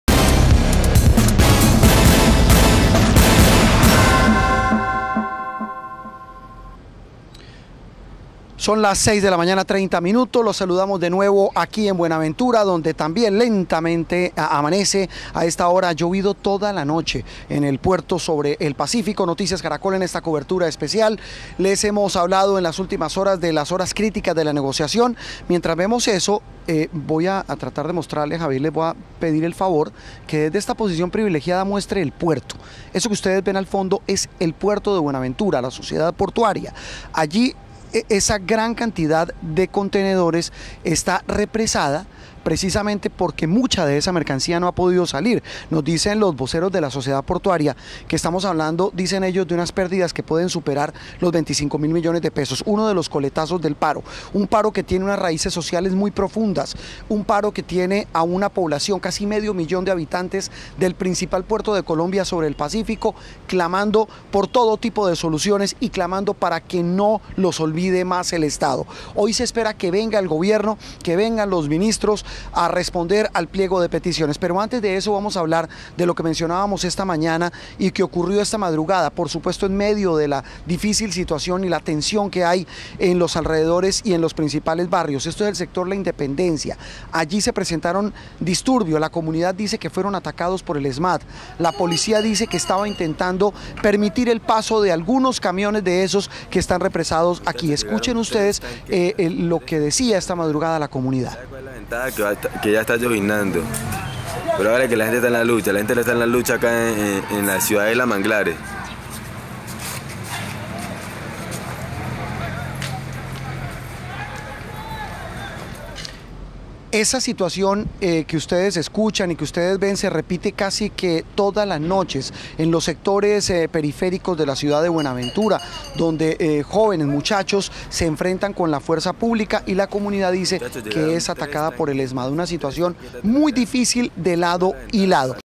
Momentos de tensión: con video
habitantes de Buenaventura denuncian ataques del Esmad